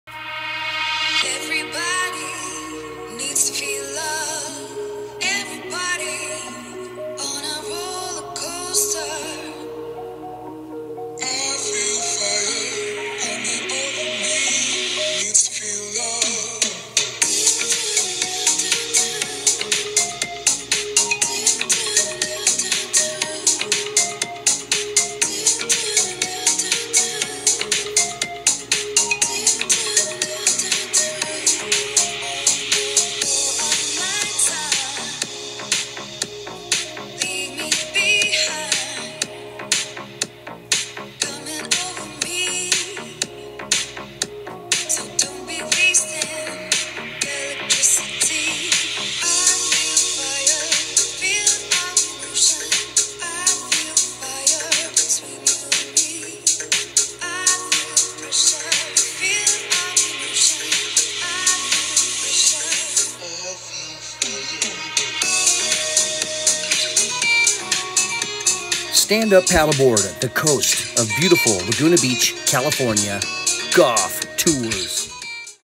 Embrace the SUP
This is Julie with her standup paddleboard Tour that was taken on the coast of beautiful Laguna Beach California, The conditions were a little choppy but still very beautiful to the eye of the beholder. 10-22-20